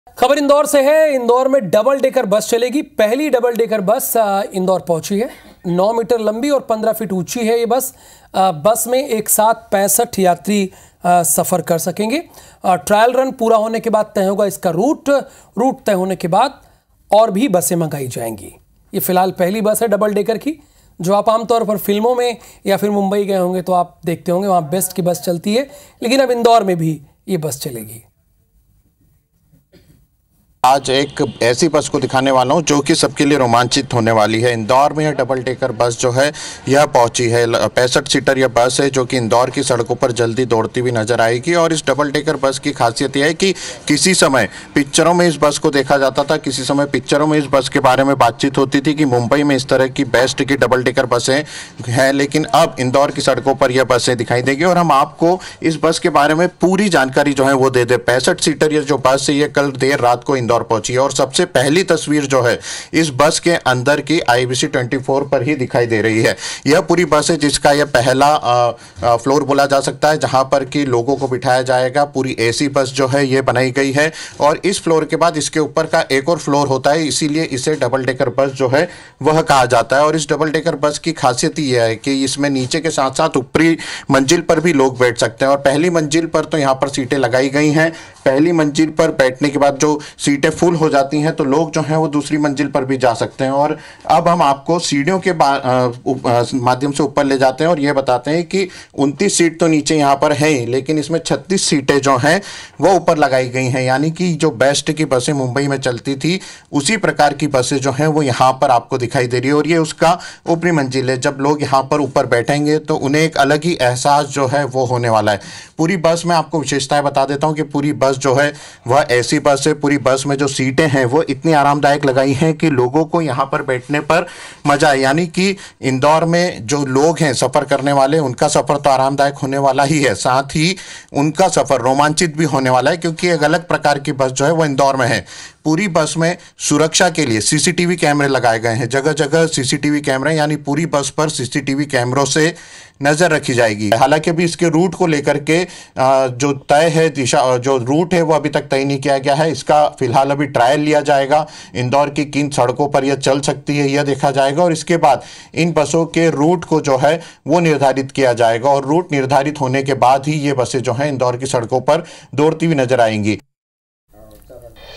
Double-Decker Buses : इंदौर में चलेंगी डबल डेकर ईवी बसें | यात्रियों को भीड़ से मिलेगी मुक्ति
IBC24, Double-Decker Buses, इंदौर में चलेंगी डबल डेकर ईवी बसें, यात्रियों को भीड़ से मिलेगी मुक्ति